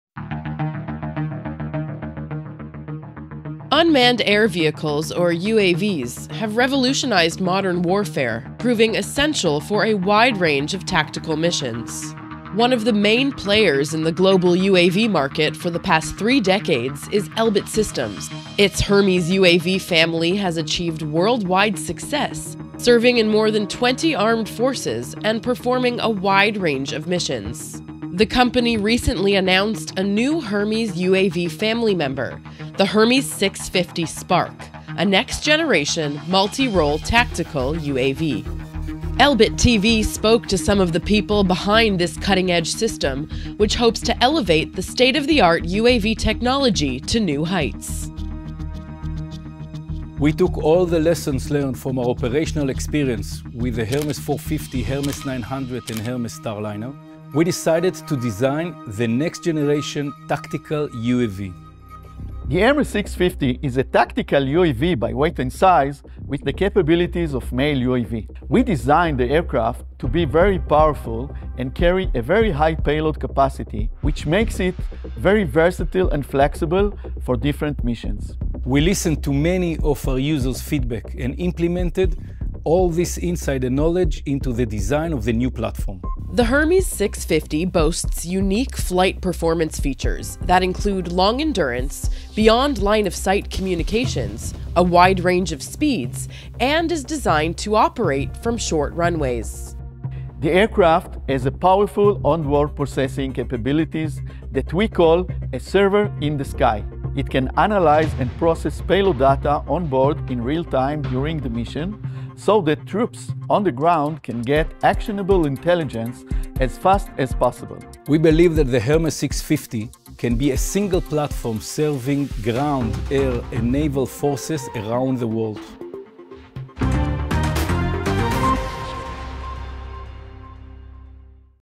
Israeli aerospace engineering firm presents the Elbit Hermes 650 SPARK.
The new UAV features a high payload capacity, long range communications, and on board server to process data while the mission is in progress, and the ability to take off from short runways, giving it the ability to support air, sea, and ground forces anywhere in the world